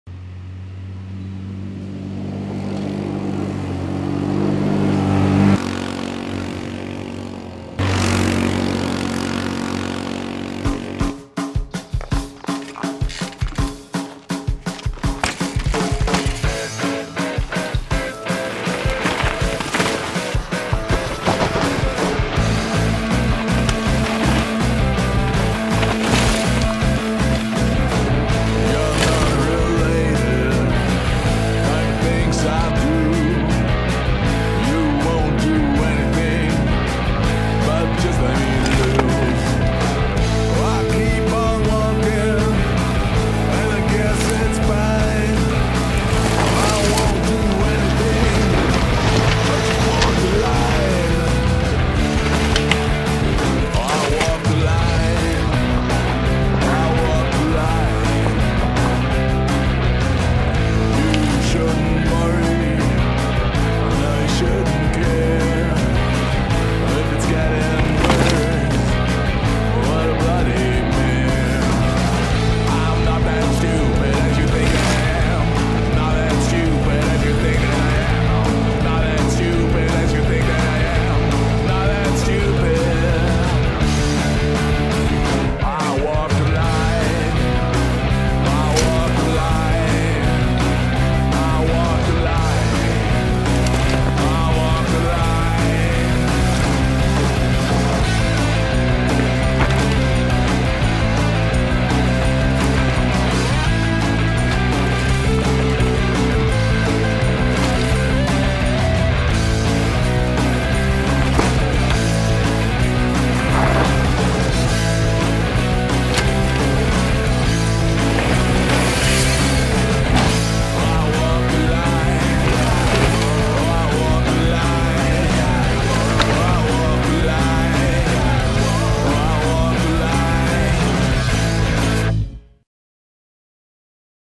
Custom DH Dream Track in Neko Mulally's Backyard!
Neko recently had the chance to design and build his very own test track with features that he would like to practice and to tune his bike for big races. The result? Bailey Mountain Bike Park in Asheville, North Carolina. With help from his friend Steve Wentz from Momentum Trail in CO, this trail has turned out to look like one hell of a good time.